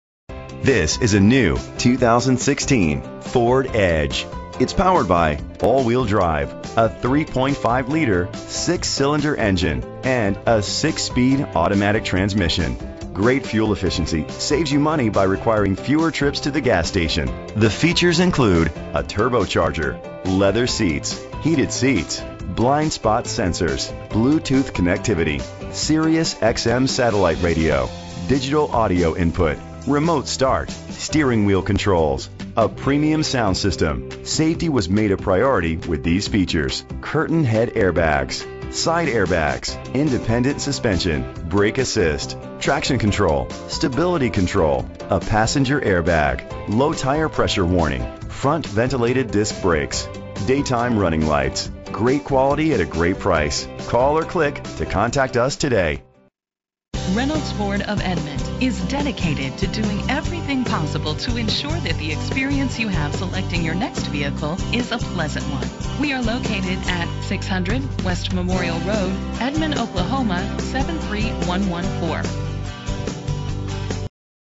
2016 Ford Edge - Edmond OK
Phone:
Year: 2016
Make: Ford
Model: Edge
Trim: SEL
Engine: 3.5 liter 6 cylinder 24 valve
Transmission: 6-Speed Automatic
Color: White
Mileage: 3
Address:
600 West Memorial Rd